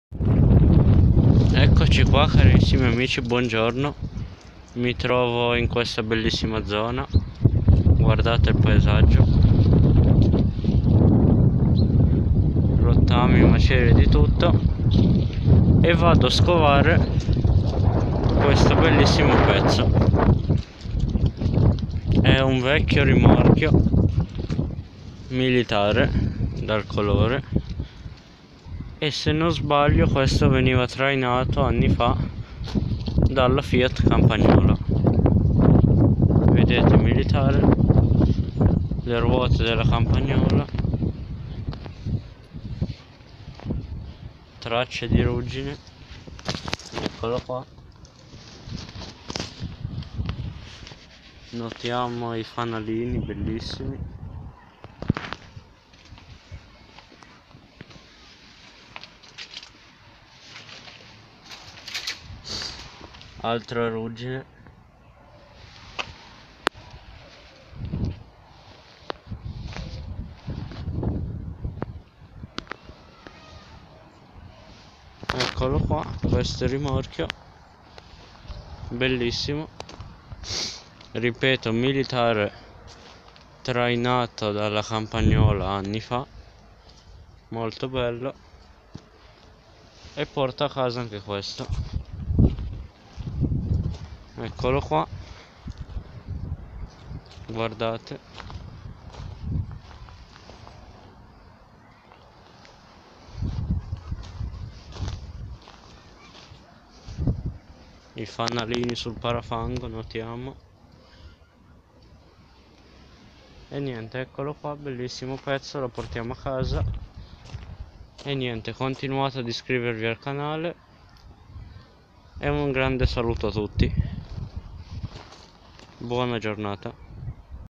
Vecchio e raro rimorchio militare d'epoca
Tranquillamente passeggiando vado a notare con grande stupore questo bellissimo rimorchio d'epoca militare trainato probabilmente da una campagnola